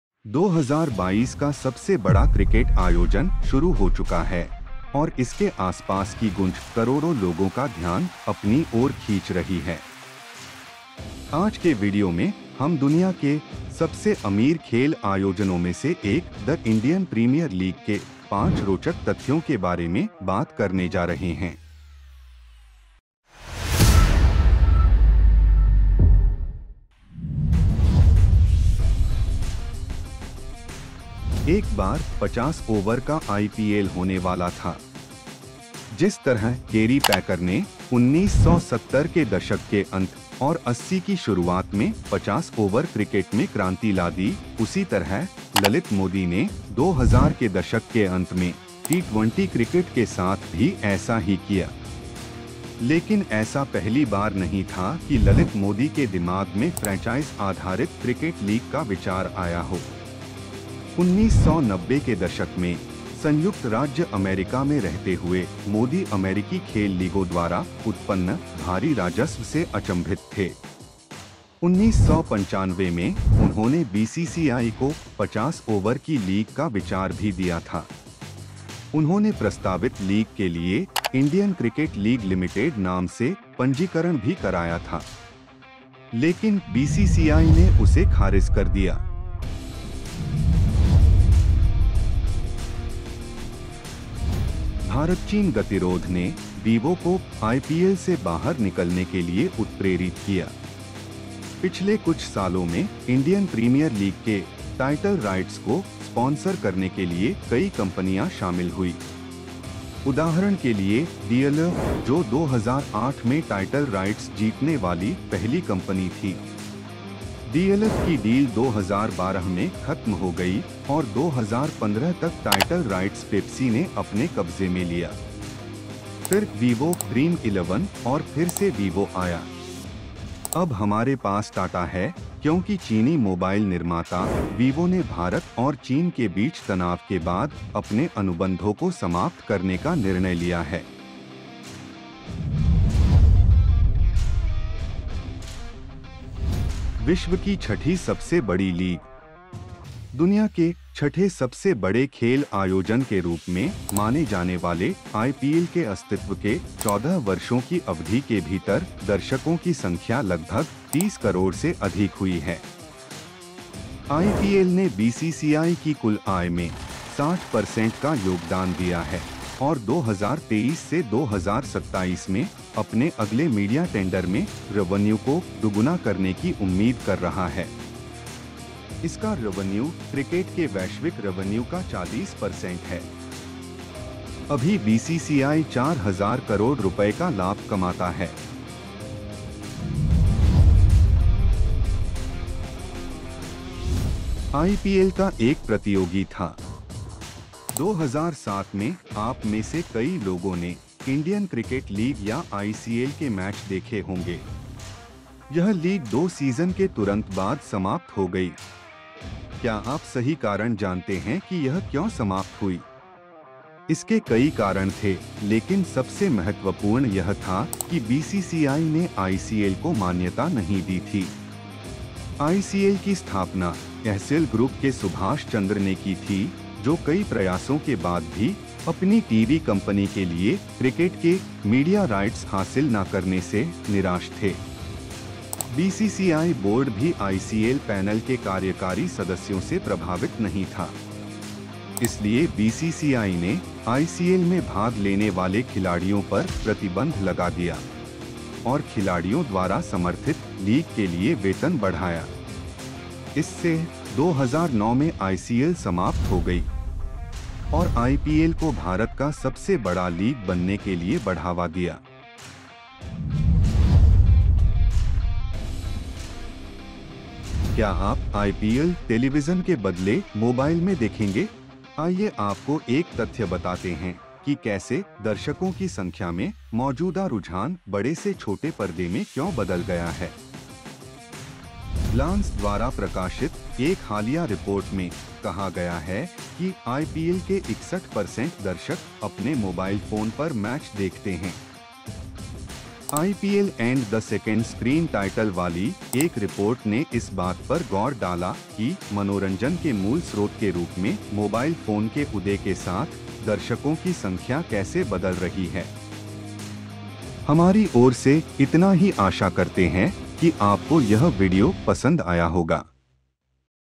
IPL के 5 UNKNOWN FACTS जो आप नहीं जानते | आईपीएल से जुड़े रोचक तथ्य
भारत की सबसे बड़ी स्पोर्टिंग इवेंट, The Indian Premier League, हमेशा अपने दर्शकों के लिए रोमांचक मैच प्रस्तुत करता है | आज के वीडियो में, हम IPL के 5 सबसे अज्ञात चीजों के बारे में बात करने जा रहे हैं |